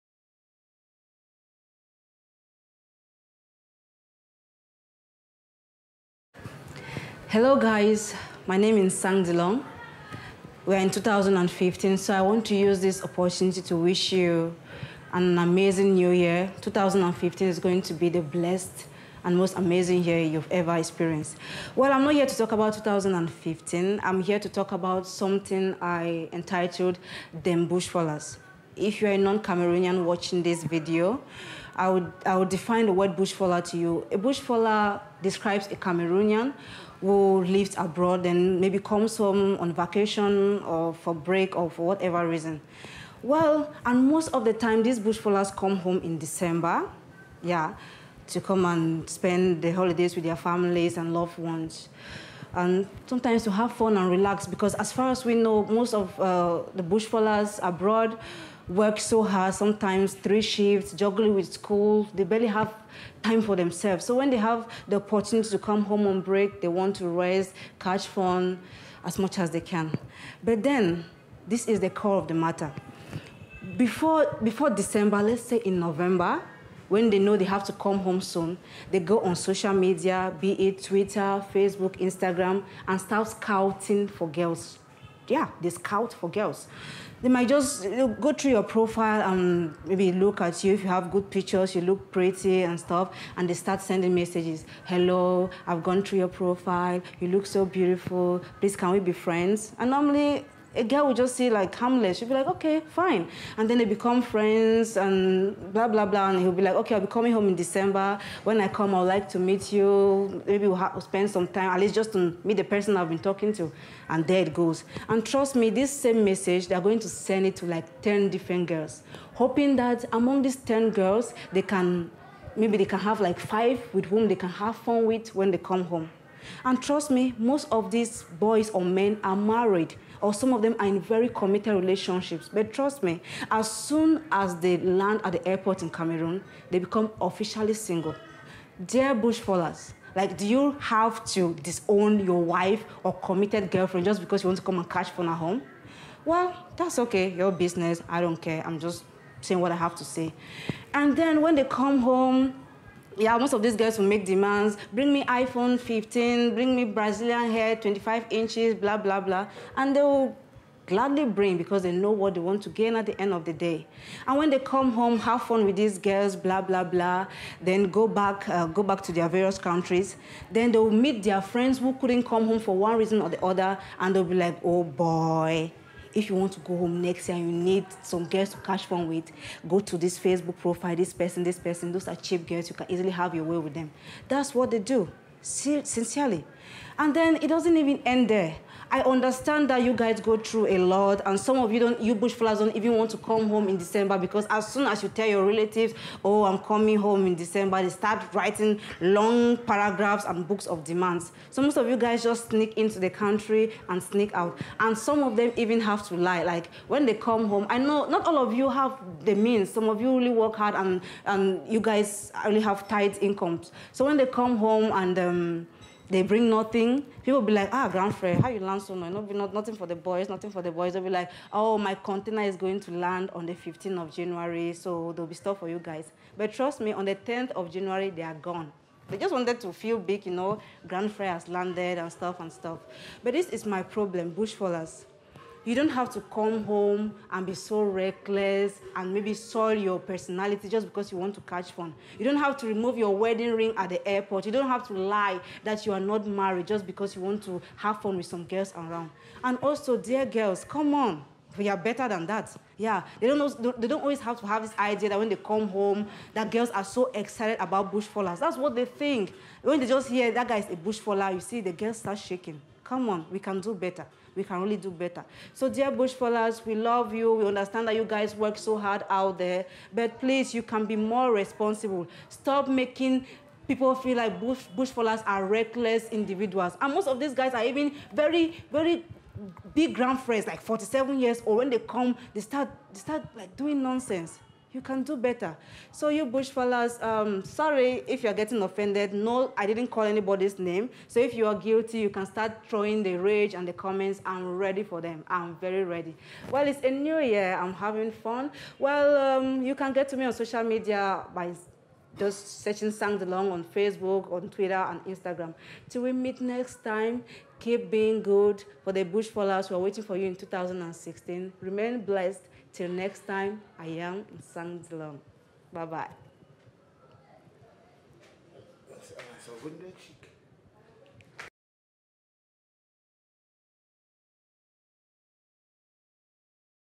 What Is A Bushfaller? by Miss West Africa Cameroon 2011 - Nsang Dilong
Not a reigning Miss West Africa Queen, but still a queen. Check out the former Miss West Africa Cameroon 2011's message to foreigners.